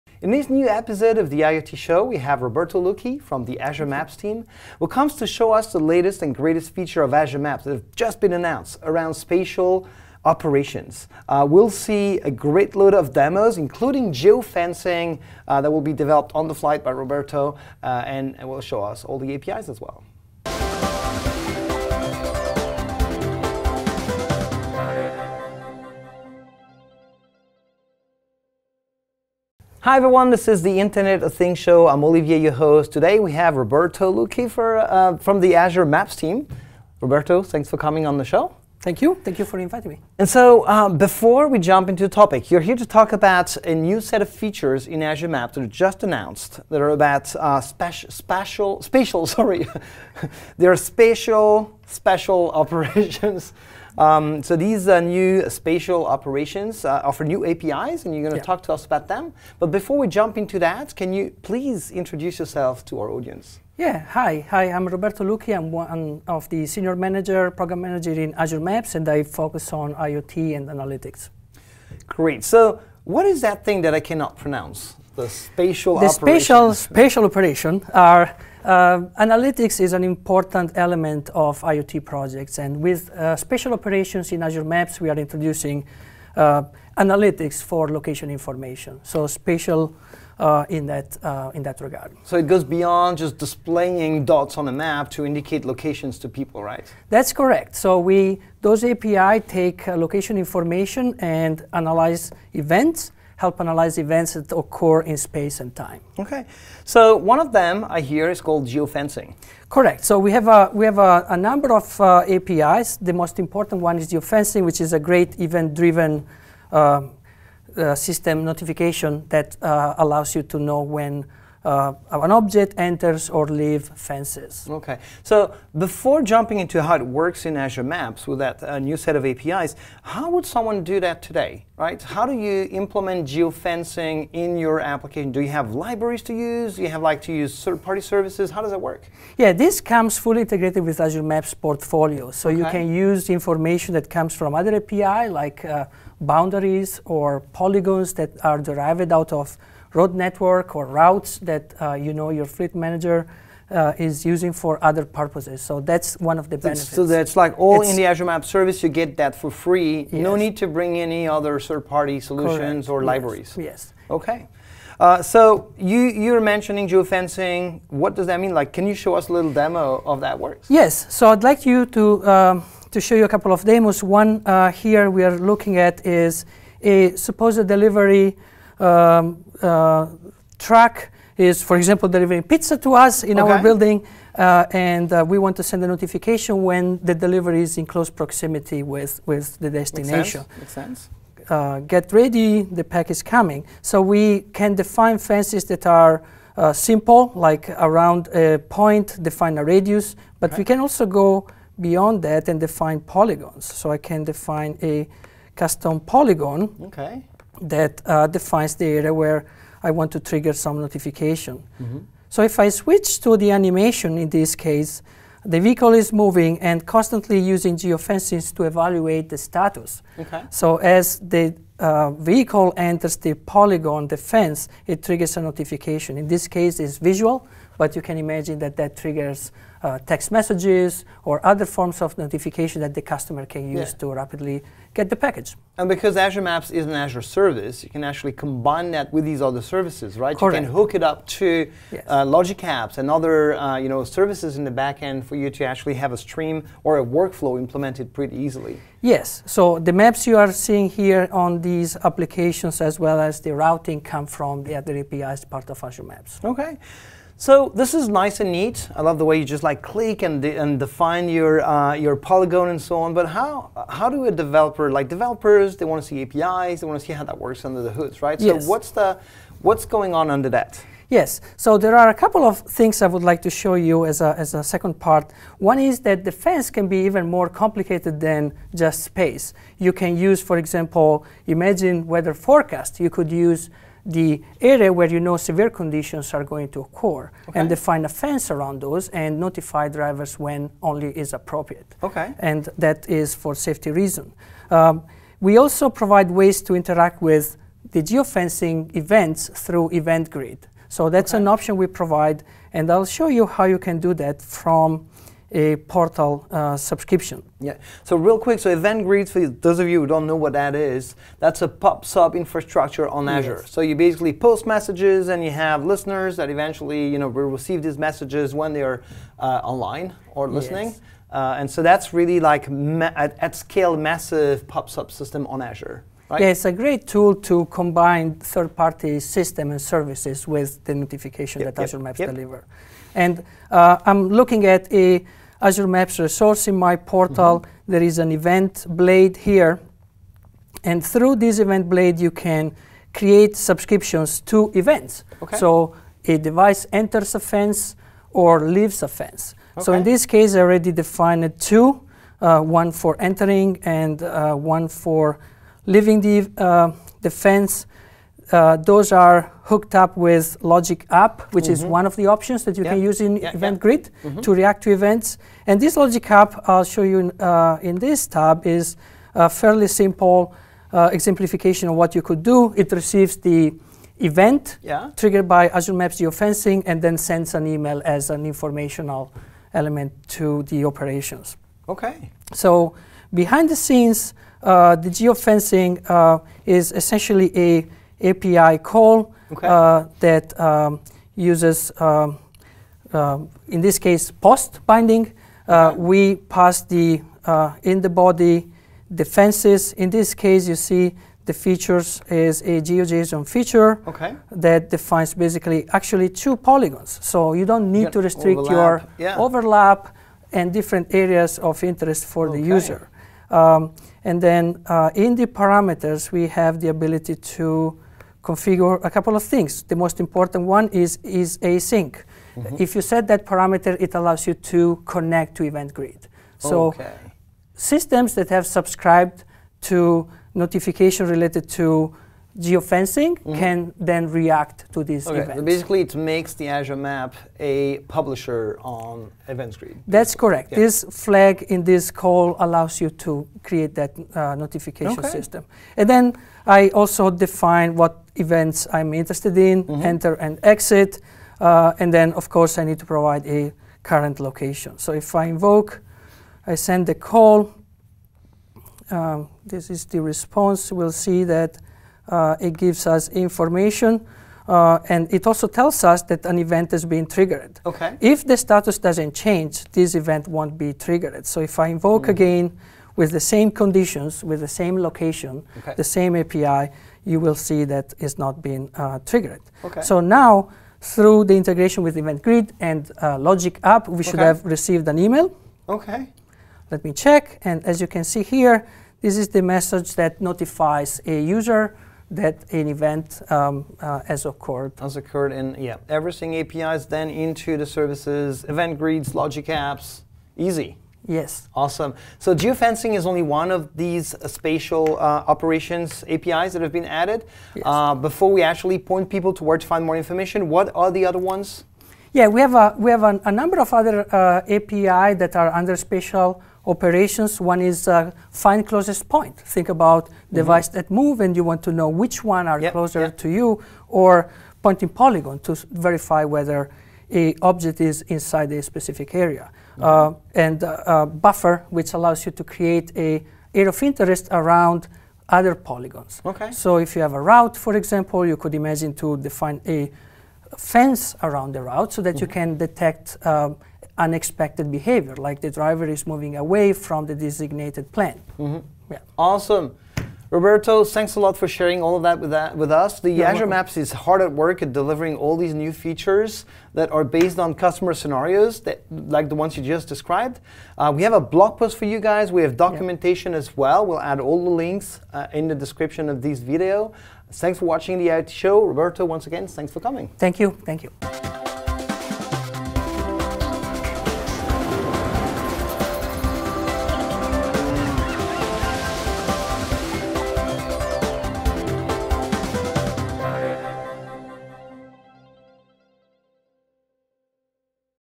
Introducing Spatial operations for Azure Maps
The ability to analyze data is a core facet of the Internet of Things. Azure Maps Spatial Operations will take location information and analyze it on the fly to help inform our customers of ongoing events happening in time and space. The Spatial Operations we are launching consist of  Geofencing, Buffer, Closest Point, Great Circle Distance and Point in Polygon. We will demonstrate geofencing capabilities, how to associate fences with temporal constraints so that fences are evaluated only when relevant, and how to react to Geofence events with Event Grid. Finally, we will talk about how other spatial operations can support geofencing and other scenarios.

Tutorial on how to setup a geofence using Azure Maps
Tune in on March 6th on the IoT Deepdive: Location Intelligence for Transportation with Azure Maps